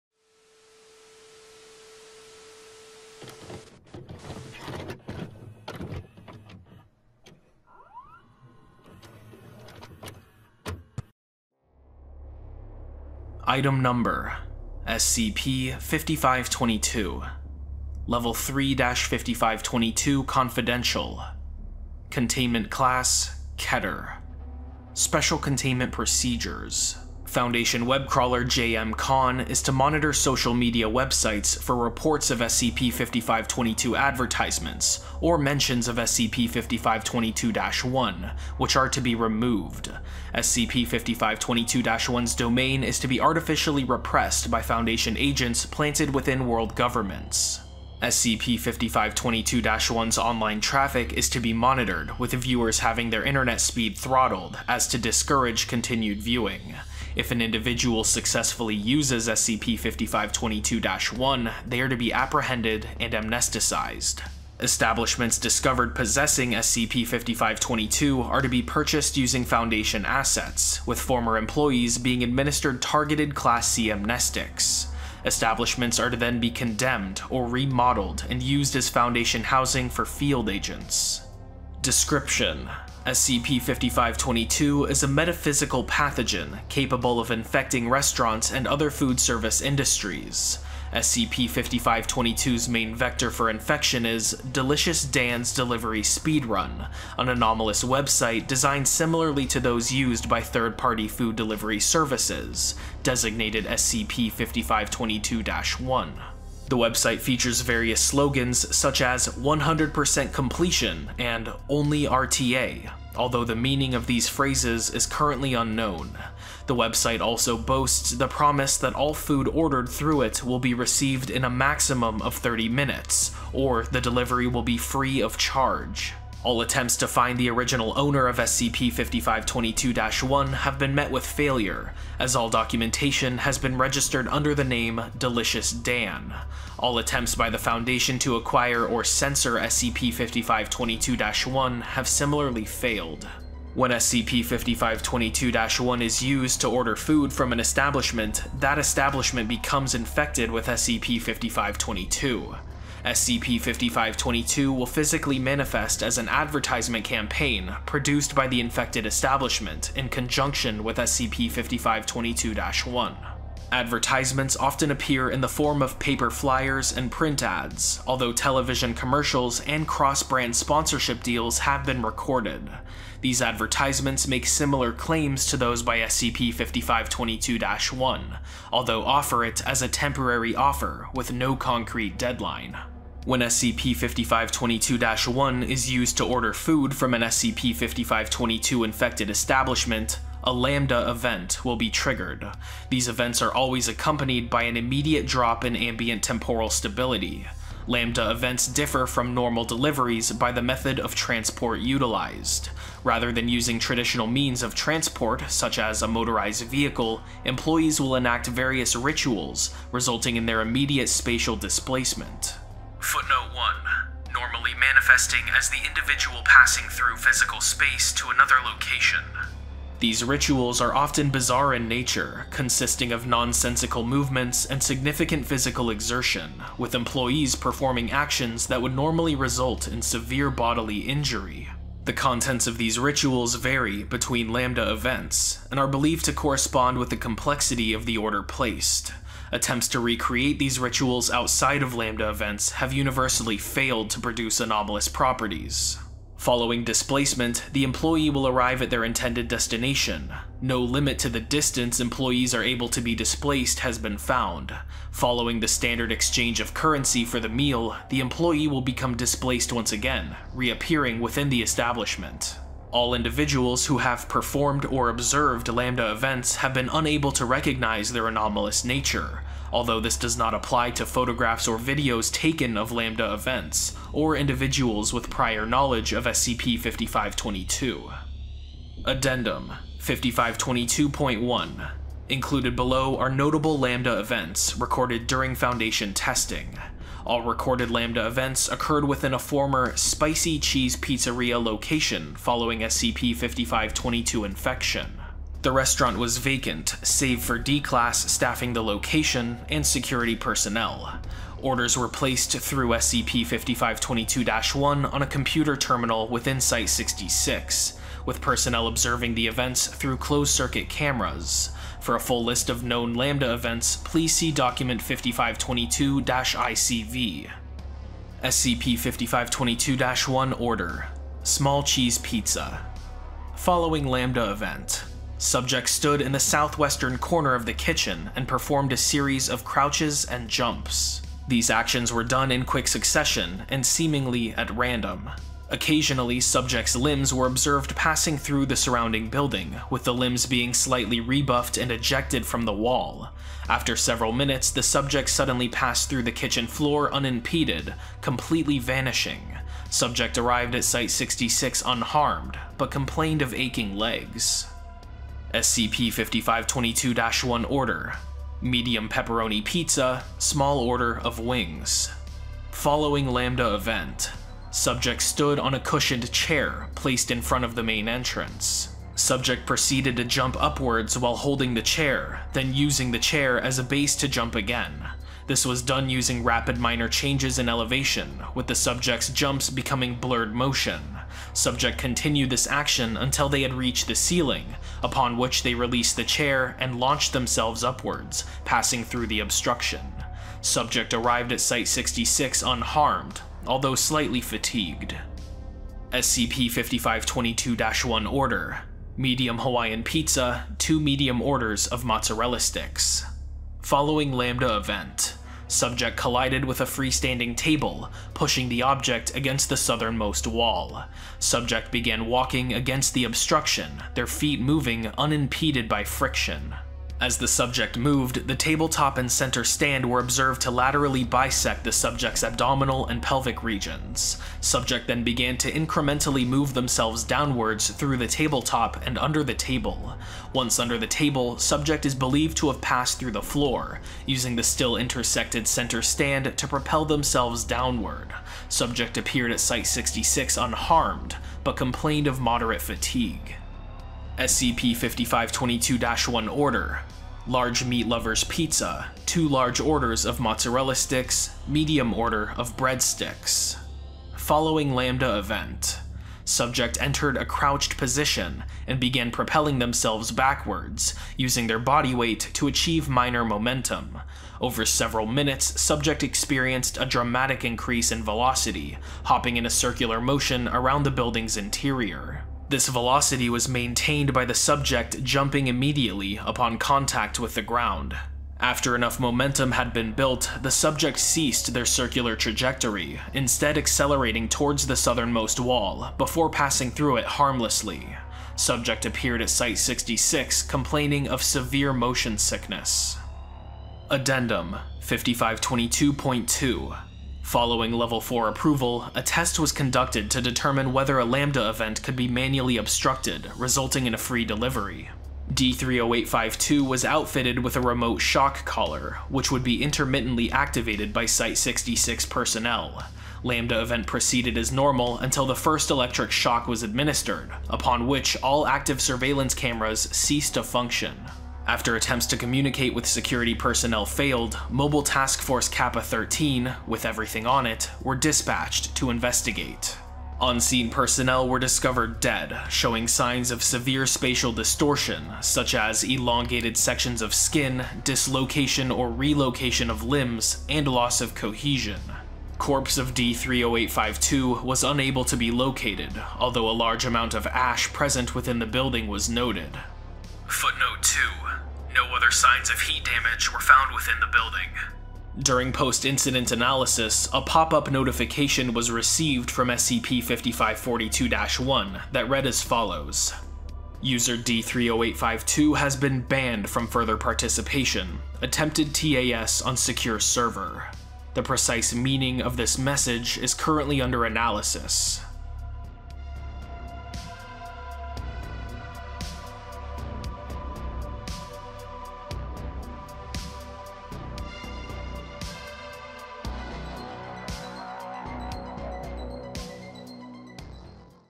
SCP-5522 | Pizza Delivery Speedrun (RTA) 100% Completion | Keter Class | Food Speedrunning SCP
Rather than using traditional means of transport such as a motorized vehicle, employees will enact various rituals resulting in their immediate spatial displacement. These rituals are often bizarre in nature, consisting of nonsensical movements and significant physical exertion, with employees preforming actions that would normally result in severe bodily injury.

SCP-5522 was written by Deadly Bread - see their author